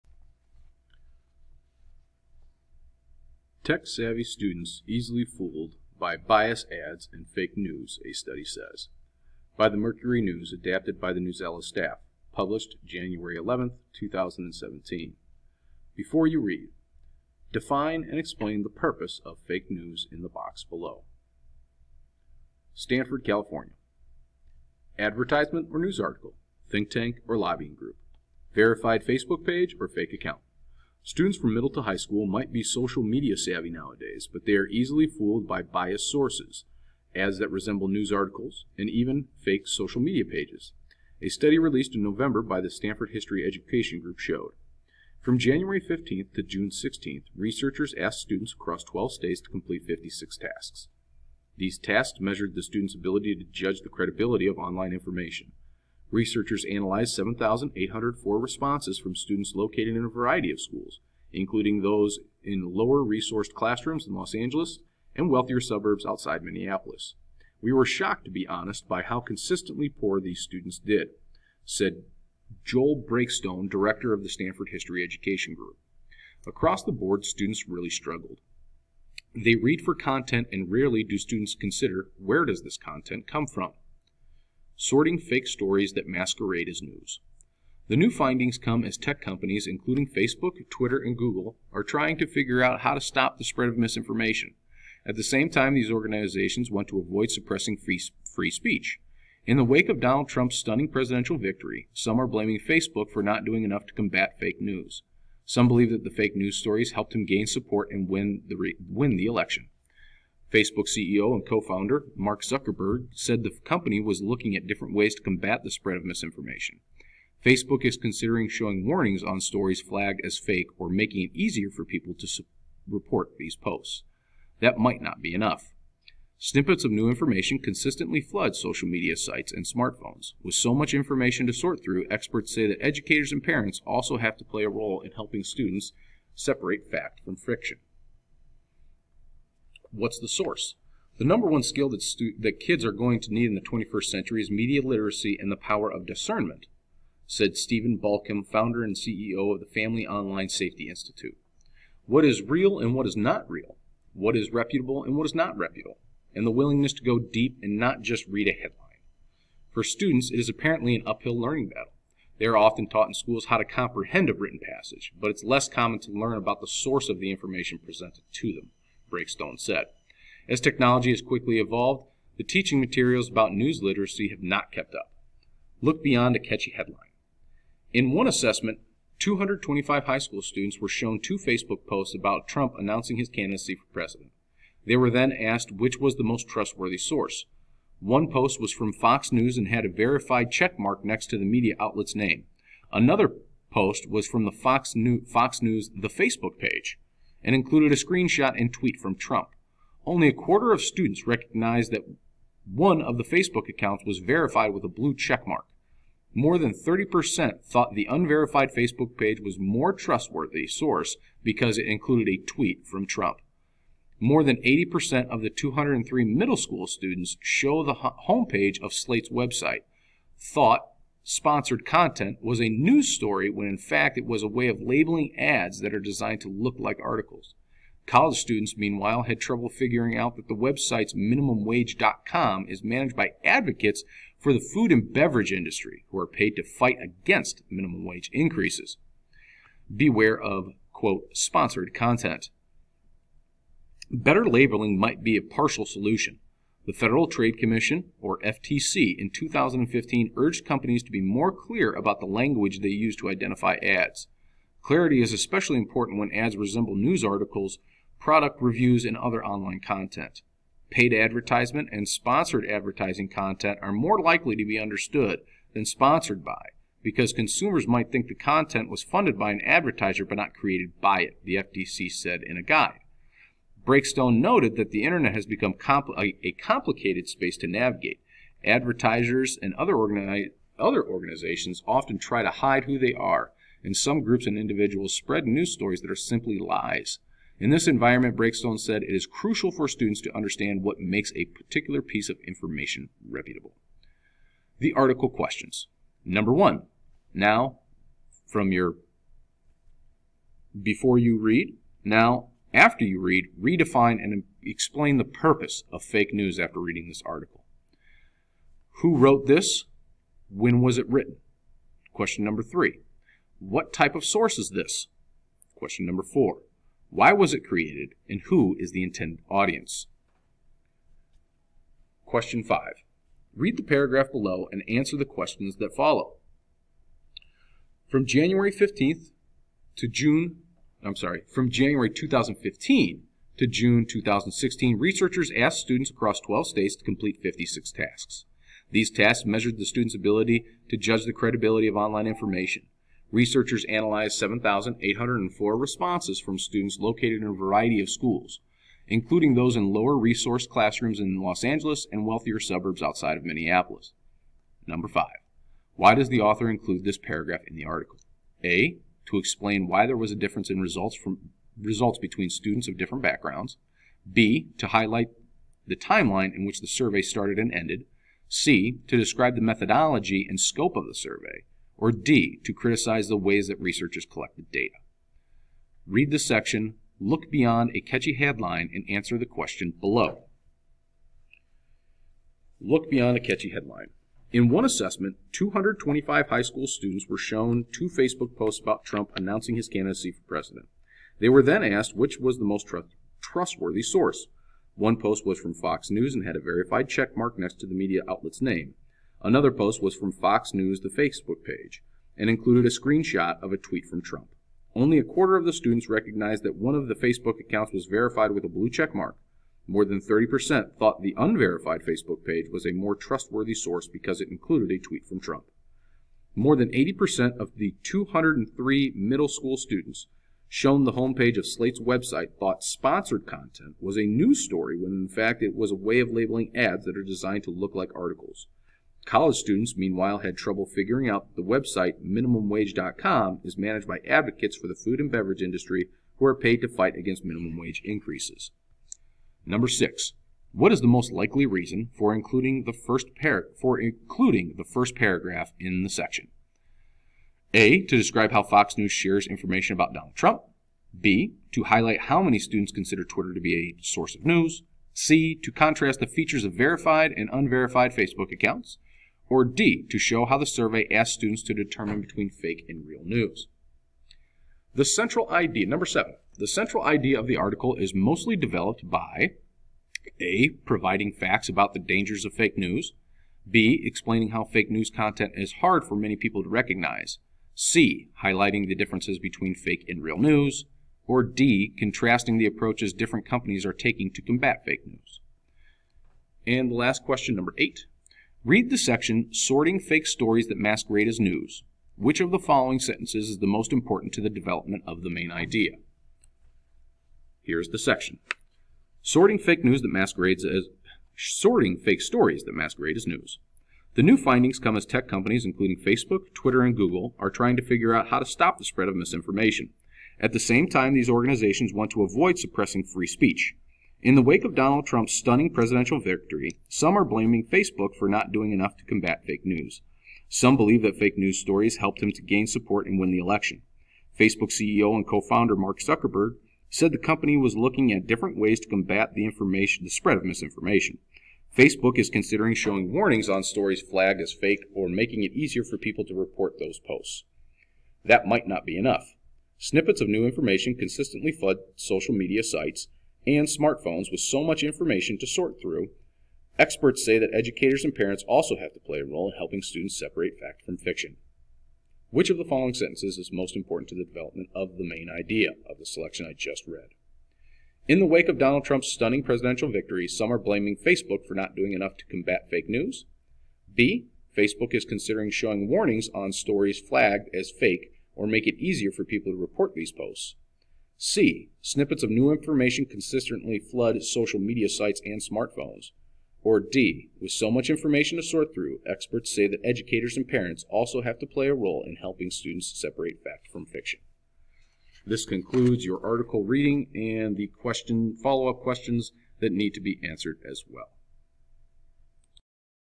Fake News Article
This recording is for  both orange and blue group...make sure you answer the questions at the end of the reading.  Please see your google classroom page for instructions.